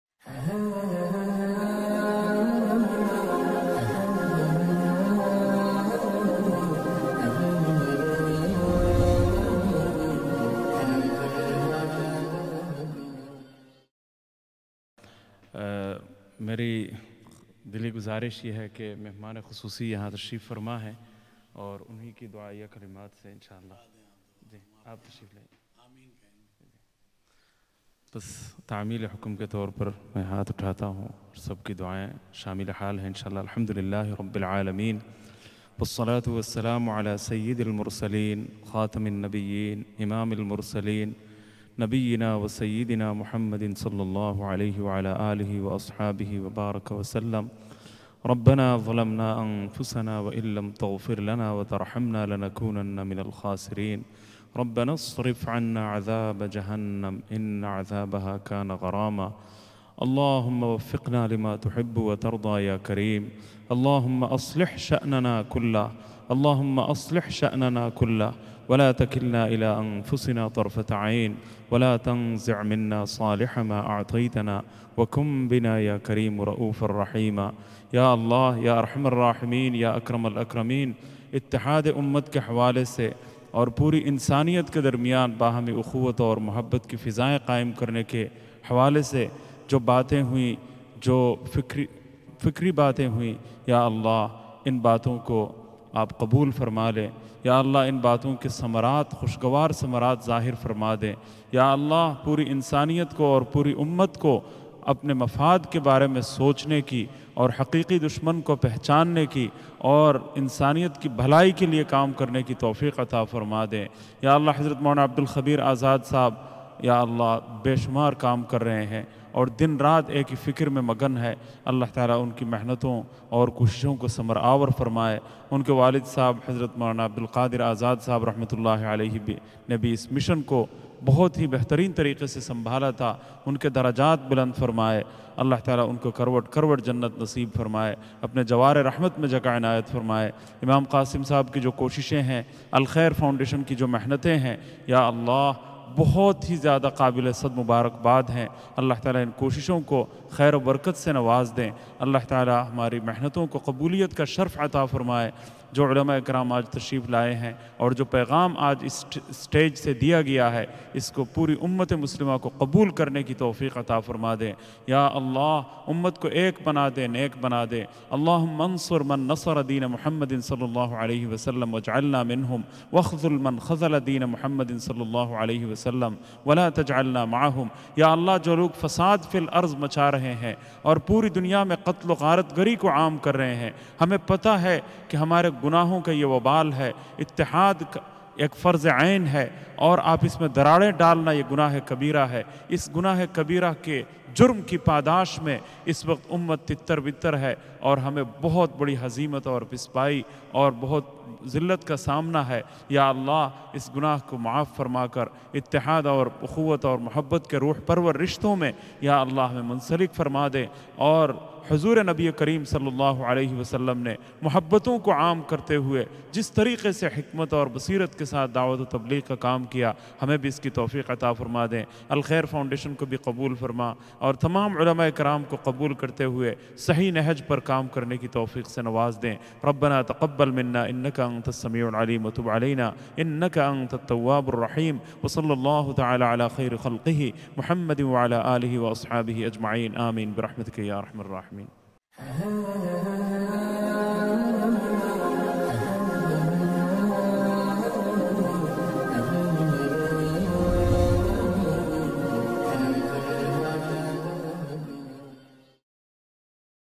IQRA TV | ULEMA & SCHOLARS | EXCLUSIVE | MUFTI ABDUL MUNTAQIM | MOULANA SYED MUHAMMAD KHABEER AZAD
In this exclusive session of "Ulema & Scholars" on IQRA TV, esteemed speakers Moulana Syed Muhammad Khabeer Azad and Mufti Abdul Muntaqim come together to share their insights on pressing contemporary issues facing the Muslim community. This engaging discussion explores themes of spirituality, community engagement, and the importance of knowledge in everyday life. Viewers will gain valuable perspectives on how to navigate modern challenges while staying rooted in faith. Join us for an enlightening conversation that inspires deeper understanding and connection within the community.